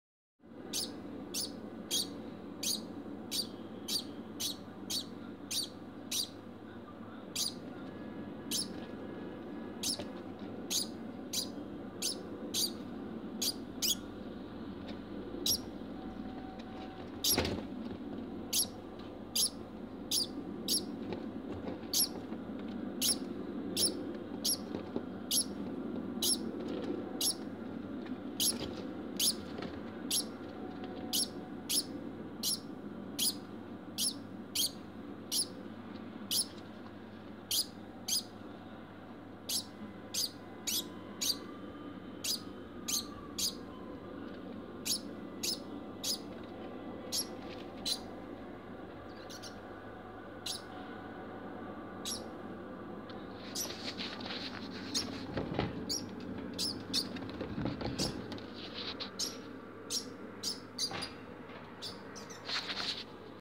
Degu Barking (post mating)
A minute of my degu (bambam) barking after mating.